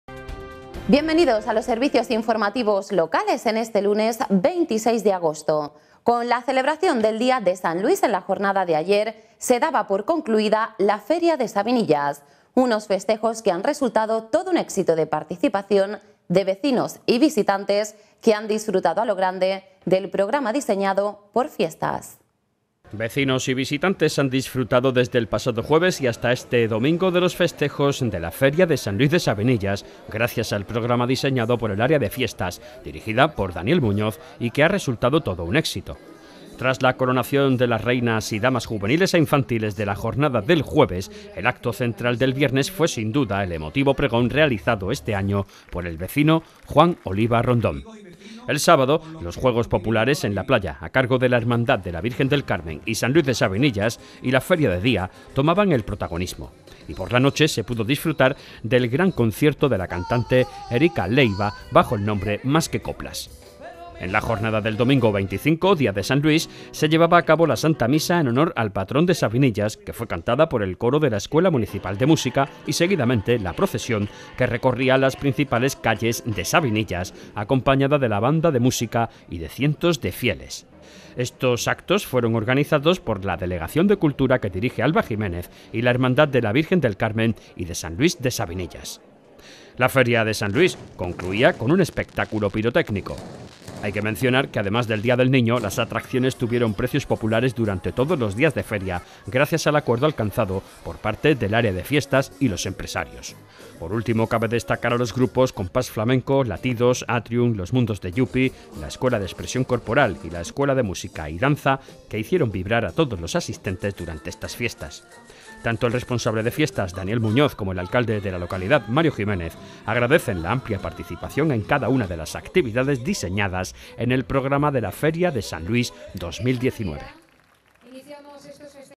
GRAN ÉXITO DE LA FERIA DE SAN LUIS DE SABINILLAS
Vecinos y visitantes han disfrutado desde el pasado jueves y hasta este domingo de los festejos de la Feria de San Luis de Sabinillas, gracias al programa diseñado por el área de Fiestas, dirigida por Daniel Muñoz, y que ha resultado todo un éxito.
Tras la coronación de las reinas y damas juveniles e infantiles de la jornada del jueves, el acto central del viernes fue sin duda el emotivo pregón realizado este año por el vecino Juan Oliva Rondón.
El sábado, los juegos populares en la playa, a cargo de la Hermandad de la Virgen del Carmen y San Luis de Sabinillas, y la feria de día tomaban el protagonismo. Y por la noche se pudo disfrutar del gran concierto de la cantante Erika Leiva, bajo el nombre “+ que coplas”.
Y en la jornada del domingo 25, día de San Luis, se llevaba a cabo la Santa Misa en honor al Patrón de Sabinillas, que fue cantada por el Coro de la Escuela Municipal de Música y seguidamente, la procesión, que recorría las principales calles de Sabinillas, acompañada de la Banda de Música y de cientos de fieles. Estos actos fueron organizados por la Delegación de Cultura, que dirige Alba Jiménez, y la Hermandad de la Virgen del Carmen y de San Luis de Sabinillas.
La feria de San Luis concluía con un espectáculo pirotécnico.
Hay que mencionar, que además del día del niño, las atracciones tuvieron precios populares durante todos los días de feria, gracias al acuerdo alcanzado por parte del área de Fiestas y los empresarios.
Por último cabe destacar a los grupos Compás flamenco, Latidos, Atrium, Los mundos de Yupi, La Escuela de Expresión Corporal y la Escuela de Música y Danza, que hicieron vibrar a todos los asistentes durante estas fiestas.
Tanto el responsable de Fiestas, Daniel Muñoz, como el alcalde de la localidad, Mario Jiménez, agradecen la amplia participación en cada una de las actividades diseñadas en el programa de la Feria de San Luis 2019.